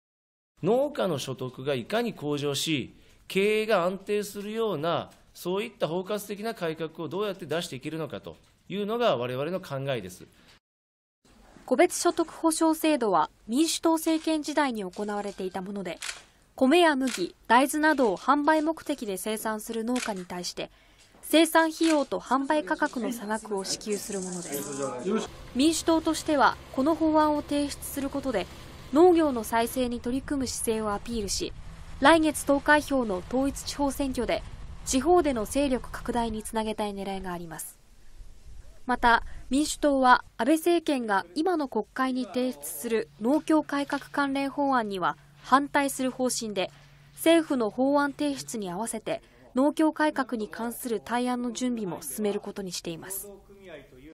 「戸別所得補償制度」復活させる法案、民主党が提出(15/03/27)
民主党は、政府が提出する農協改革法案に対抗して、戸別所得補償制度を復活させる法案を国会に提出しました。